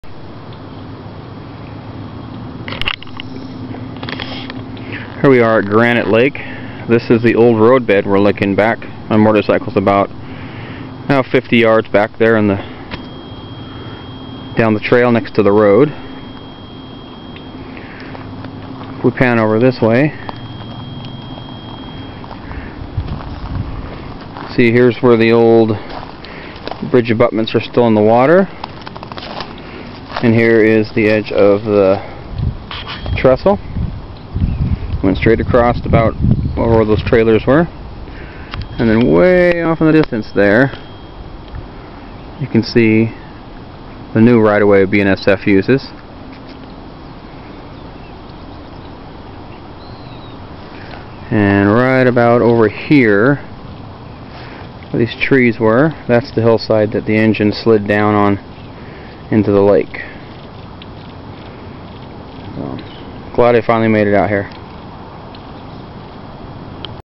Northern Pacific Granite Lake Accident Site
Located just north of Athol idaho. The old mainline for the NP went over this trestle until a wreck in the early 60's cause a new route as seen in the background. Just the engineers were killed at 79 miles an hour which was a miracle. The spot is overgrown now and there is a cabin there but it is still a very cool piece of railroad history.